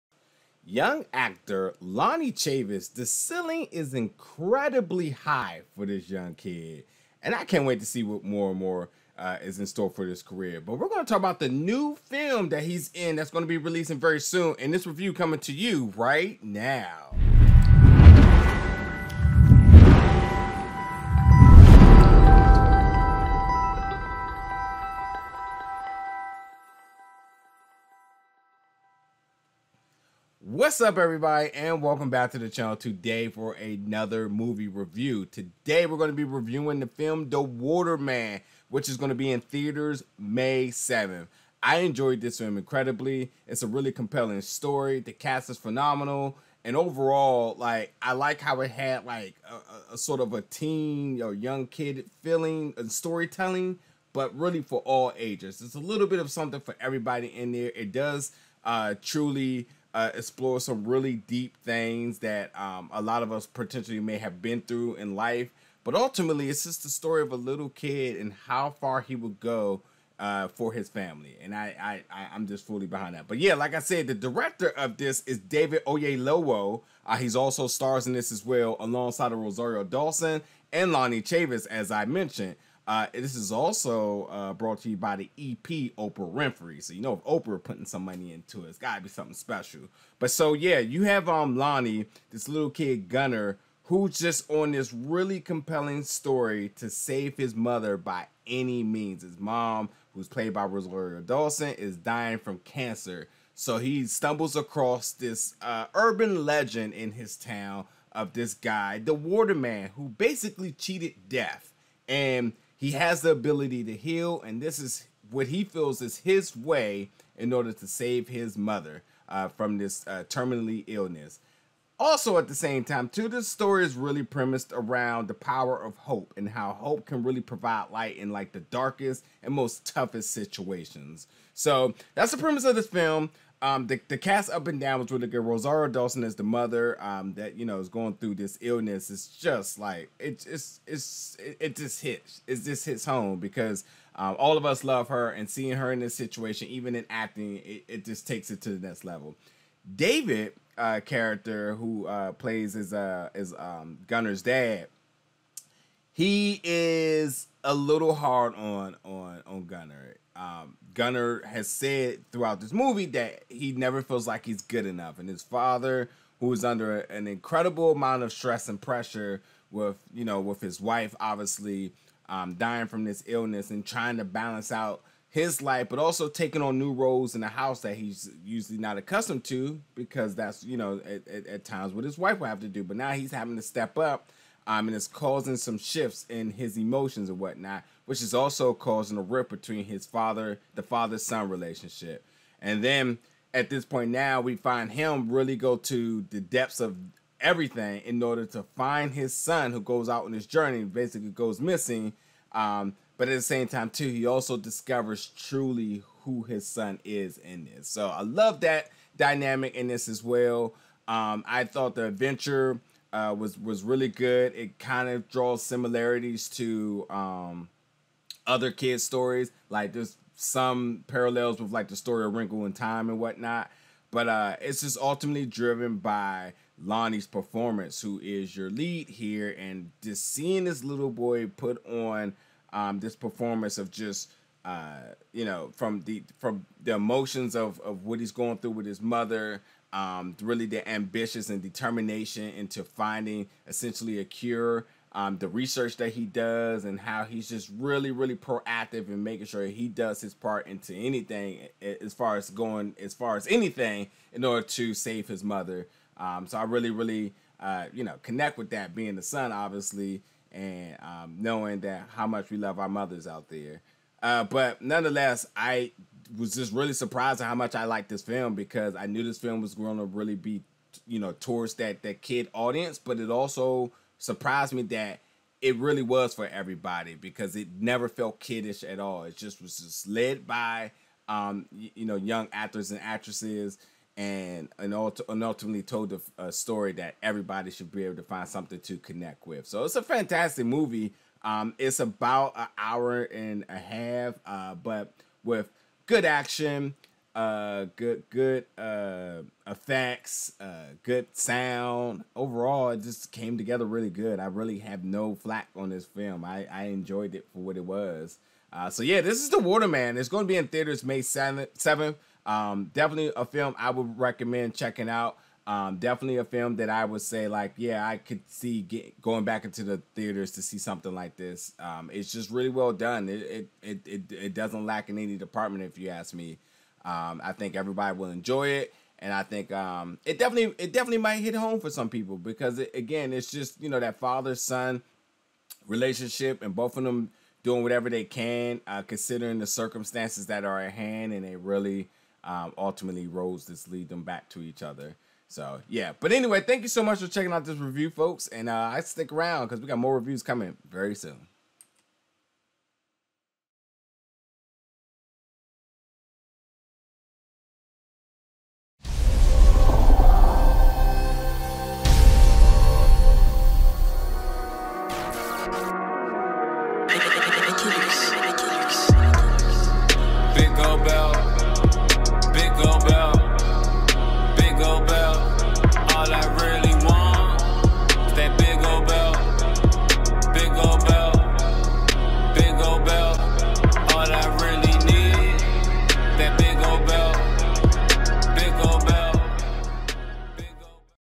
The Water Man - Movie Review (2021) | Rosario Dawson, David Oyelowo
Big Gold Belt Media review of the movie 'The Water Man' starring David Oyelowo, Rosario Dawson & Lonnie Chavis

Director  David Oyelowo

This Movie synopsis: A boy sets out on a quest to save his ill mother by searching for a mythic figure said to have magical healing powers.

The Water Man - Movie Review